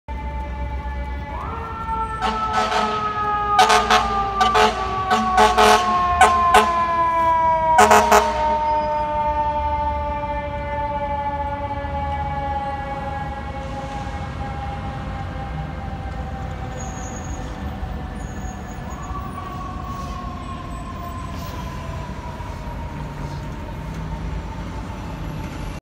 FDNY ENGINE 40 RESPONDING ON AMSTERDAM AVENUE ON THE WEST SIDE AREA OF MANHATTAN IN NEW YORK CITY.
“HERE YOU WILL SEE FDNY ENGINE 40 RESPONDING ON AMSTERDAM AVENUE ON THE UPPER WEST SIDE AREA OF MANHATTAN IN NEW YORK CITY.